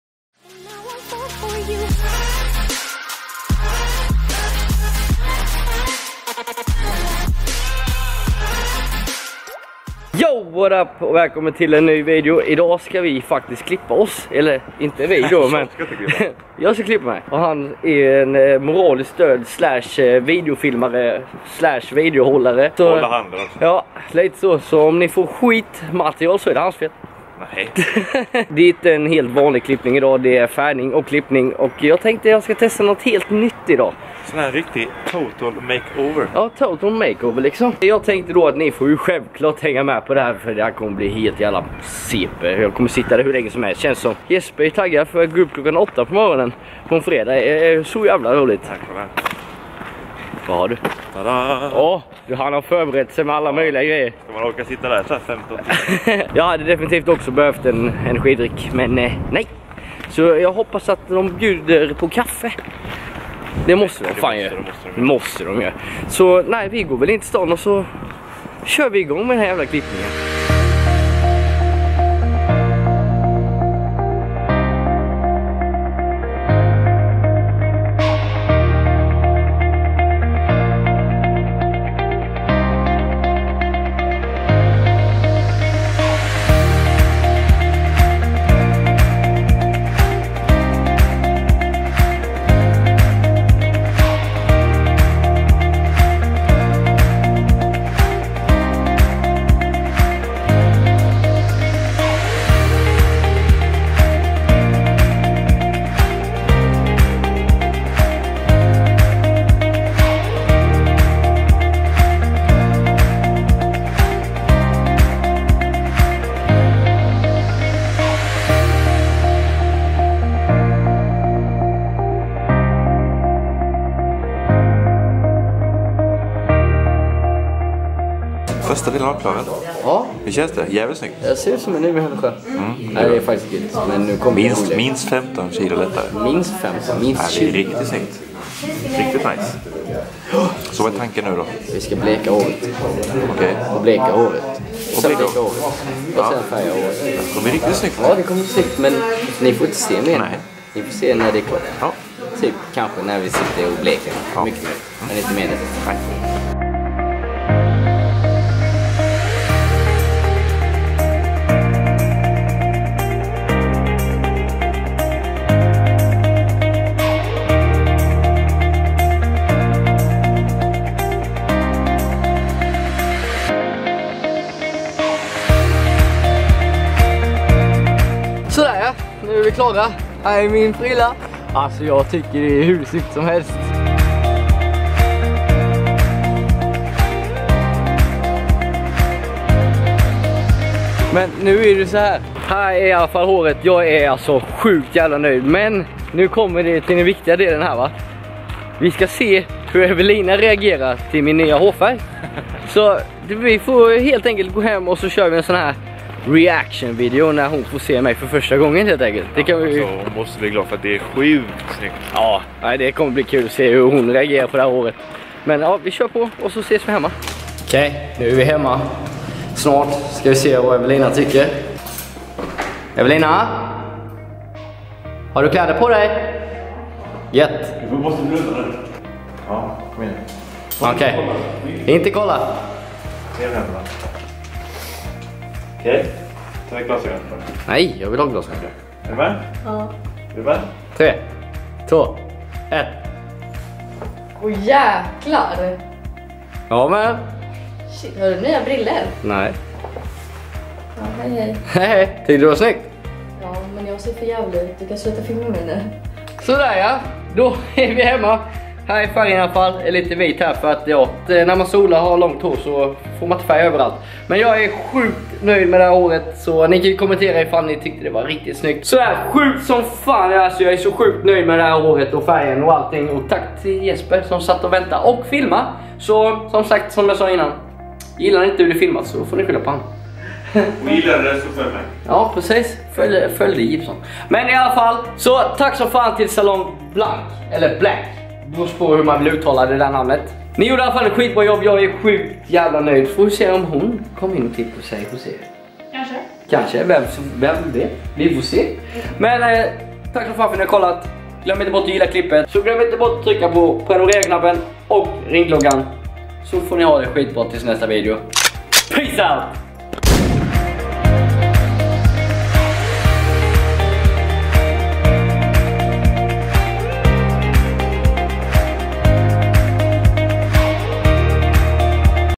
Färgar håret Silver/Blått makeover! AMAZING!
Transformation new hair color and style
New hair color made at Blanc Geraci & Lundberg
Silver and blue hair color. Total makeover and hair coloring
Klippte och färgade mitt hår Silver/blått på Blanc Geraci & Lundberg
Herregud vad snyggt det blev! man ska våga göra ovanliga grejer!
amazing hair transformation

Inte prenumerant ännu?... Herregud.. tryck på länken bara! 1 miljon here we come!